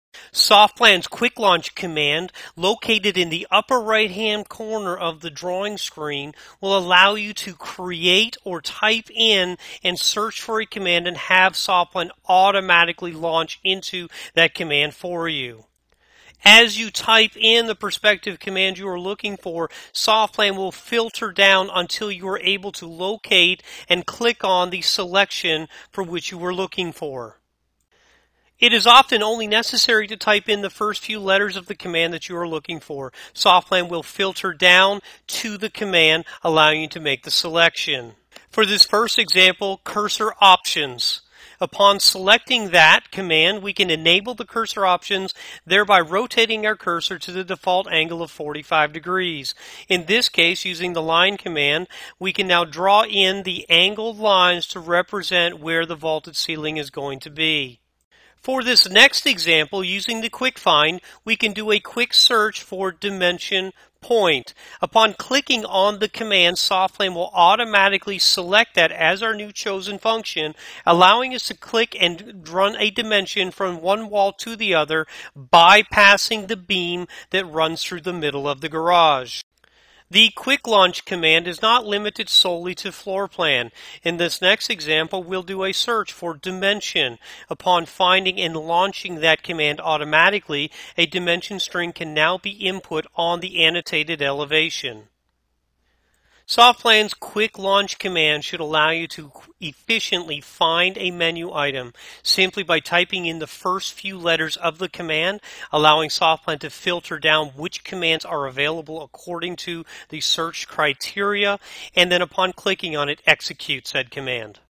Quick Launch Menu Command
The video covers the use of SoftPlan's Quick Launch Command